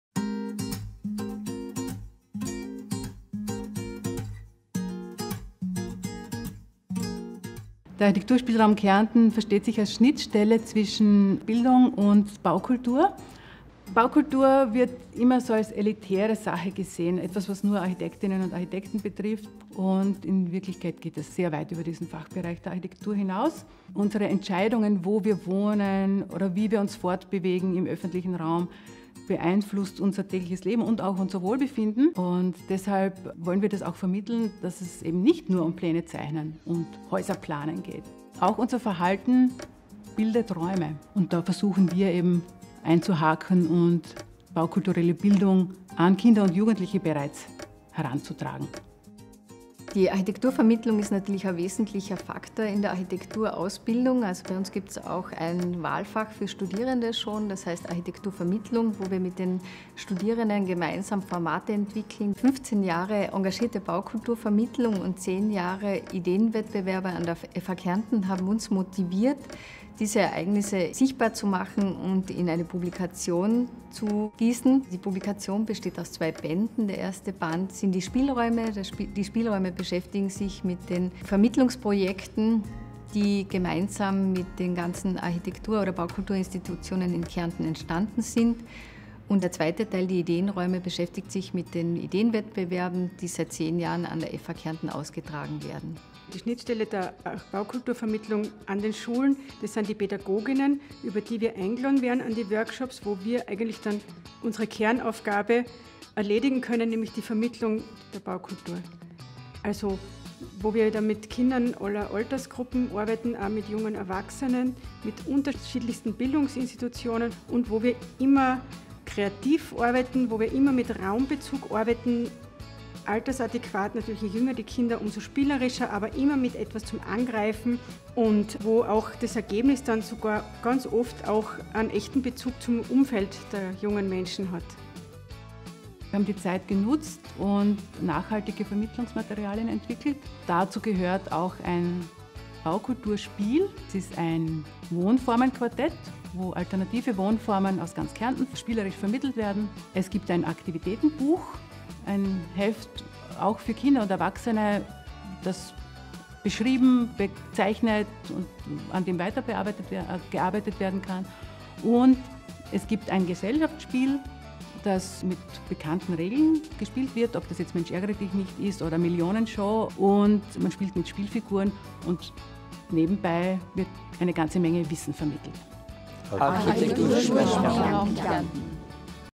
15 Jahre Architektur_Spiel_Raum_Kärnten
In Band 11 blickt der Studiengang gemeinsam mit dem ARCHITEKTUR_SPIEL_RAUM_KÄRNTEN und dem Architektur Haus Kärnten auf 15 Jahre Baukultur-Vermittlungsarbeit zurück. Präsentiert werden die beiden Publikationen und das Baukulturspiel KEIN SCHÖNER LAND.

Ein Filmbeitrag des Architektur Haus Kärnten.